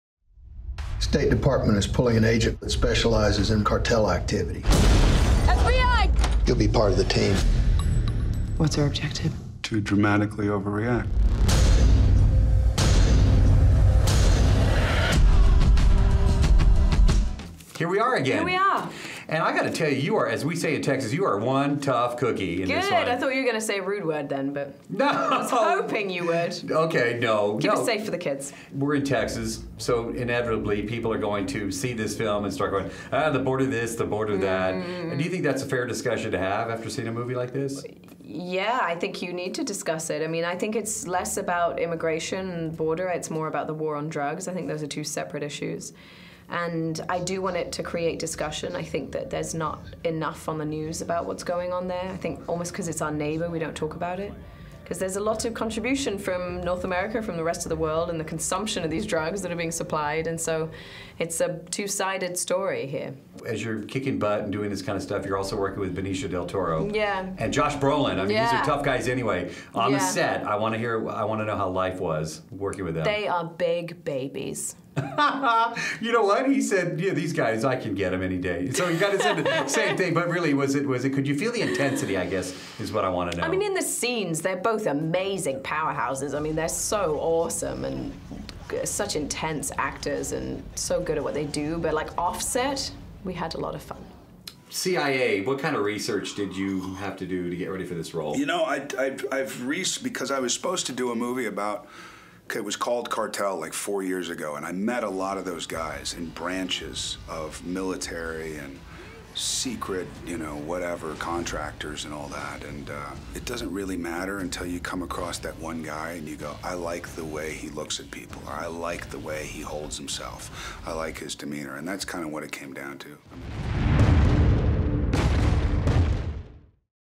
SICARIO: Emily Blunt & Josh Brolin Discuss the War on Drugs
Dave Morales sits down with Benicio Del Toro to talk about filming and playing a hitman in SICARIO, immigration and the drug war.

Synopsis:

In Mexico, SICARIO means hitman. In the lawless border area stretching between the U.S. and Mexico, an idealistic FBI agent [Emily Blunt] is enlisted by an elite government task force official [Josh Brolin] to aid in the escalating war against drugs. Led by an enigmatic consultant with a questionable past [Benicio Del Toro], the team sets out on a clandestine journey forcing Kate to question everything that she believes in order to survive.